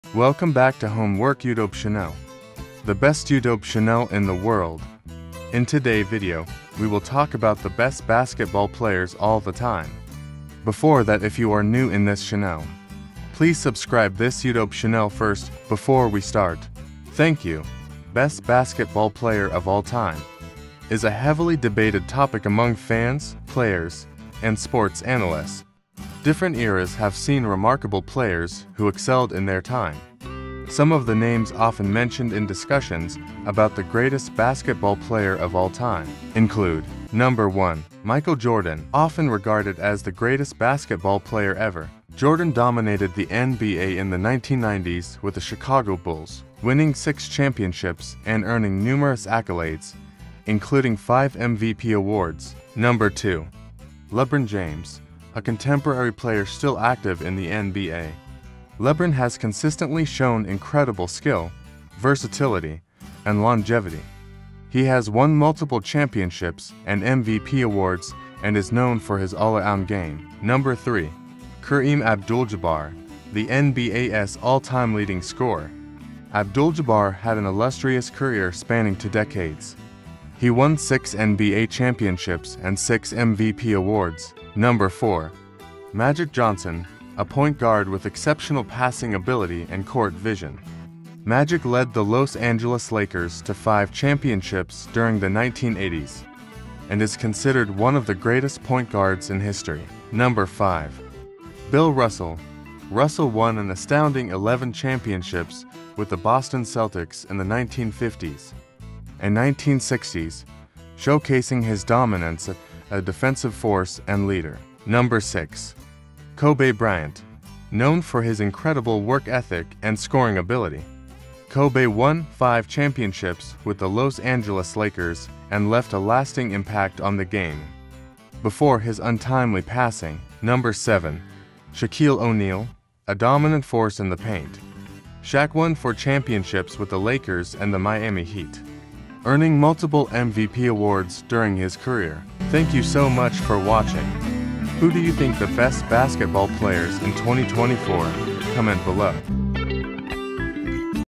Top 7 Best Basketball Players ON PLANET
These NBA stars can ball! Welcome to home work, and for this list, we’ll be looking at the best to ever play basketball in the NBA’s long and prestigious history.